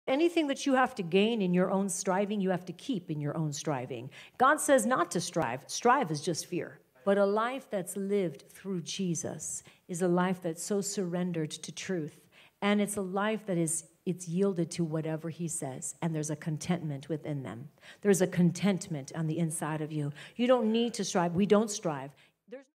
NO Striving!
Anything you have to gain in your own striving you will have to keep in your own striving.

Don’t strive stay in Him!
Fear not He is with you.

He will fill you with the wisdom and the power of God!
Trust Him!